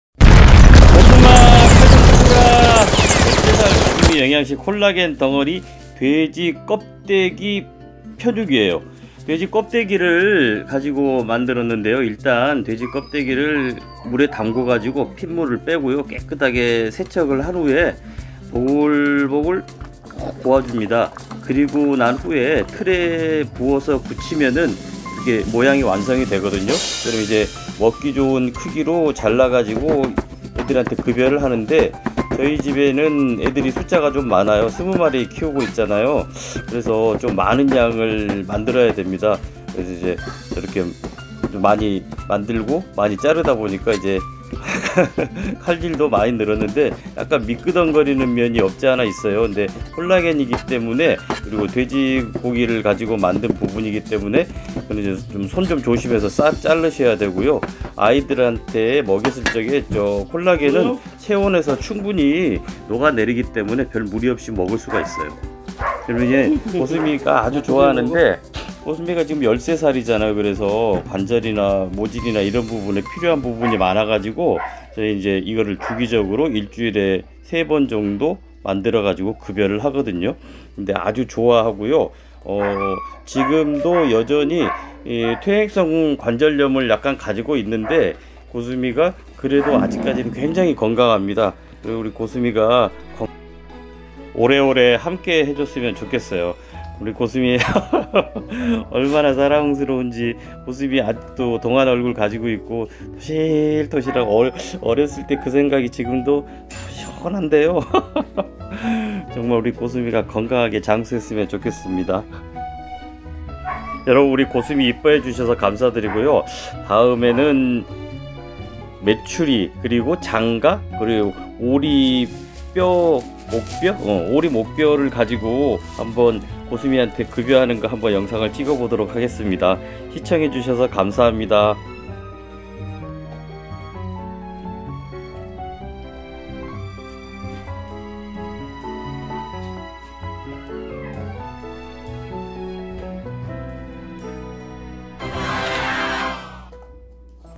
콜라겐을 먹이자 13살 말라뮤트고슴이 돼지껍데기 편육먹방
13살 노령견 고슴이는 특별한 식사가 필요합니다